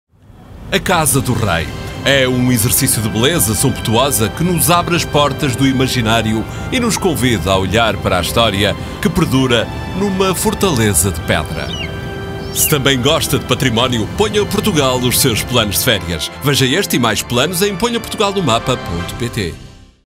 Ponha o Património no seu mapa
A História e o Património de Portugal contam mais de 9 séculos de histórias. Monumentos, castelos, ruínas estão à sua espera, no doce embalo dos dias. Ponha o património português nos seus planos de férias.